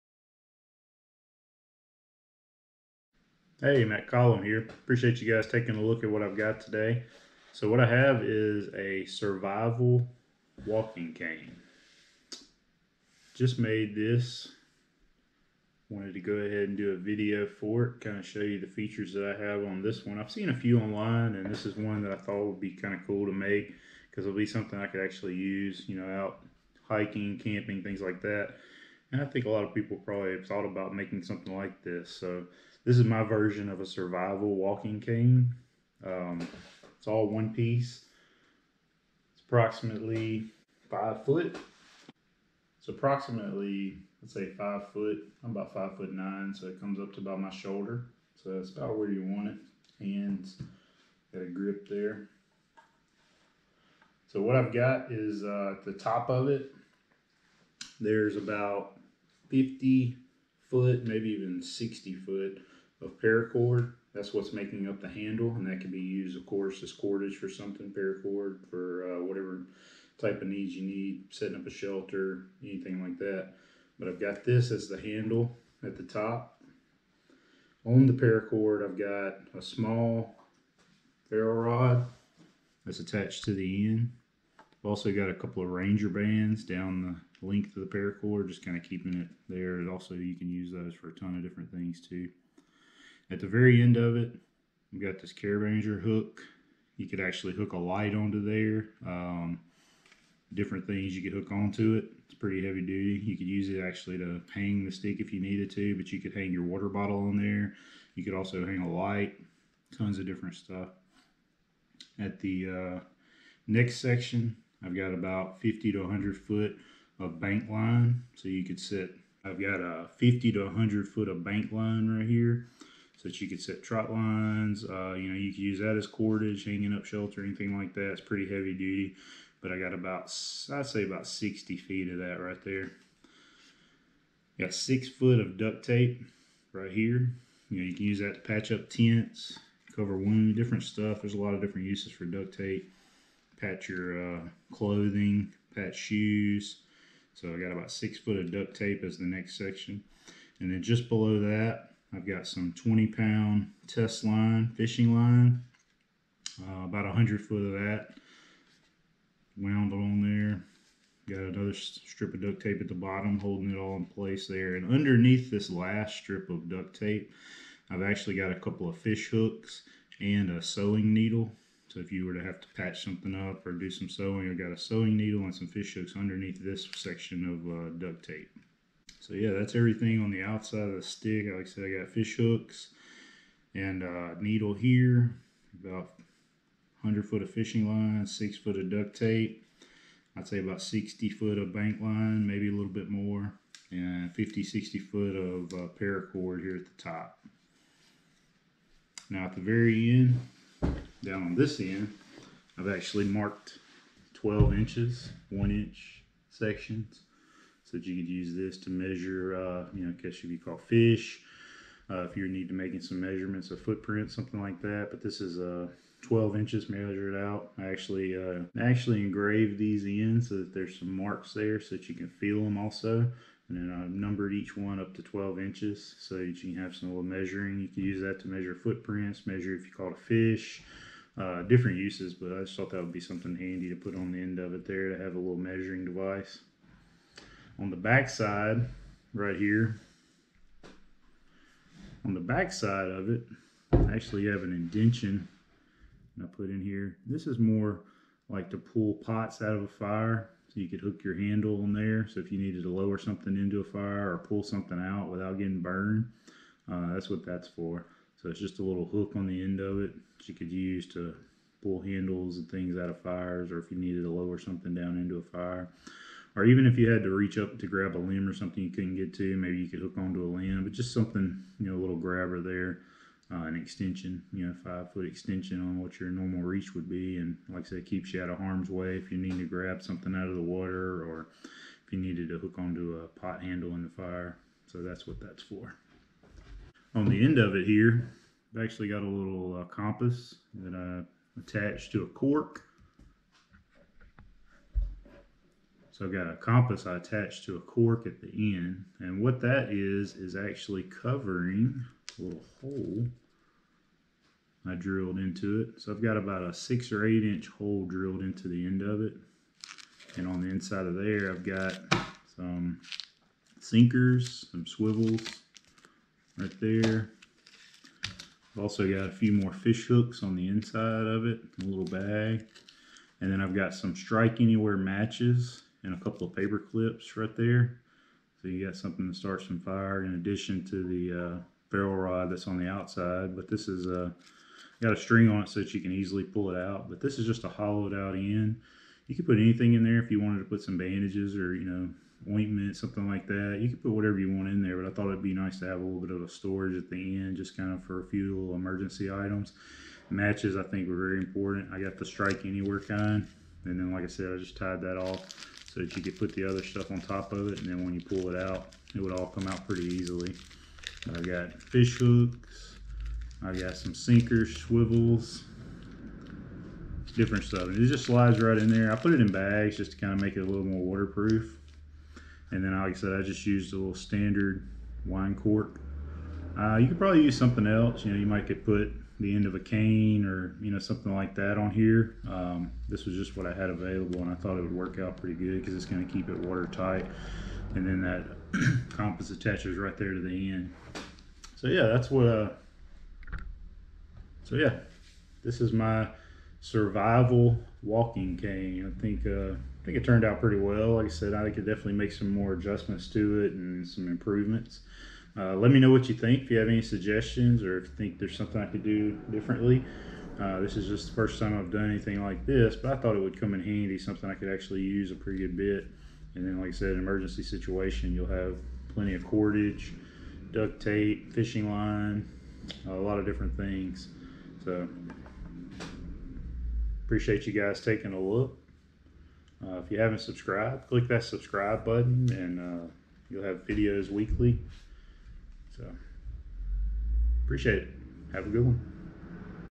Survival Walking Stick all in one survival cane great for hiking, camping, or just going for a walk
This video shows a survival walking stick that I made.  Its about 5 ft with lots of items for survival situations. Could be great for hiking or camping.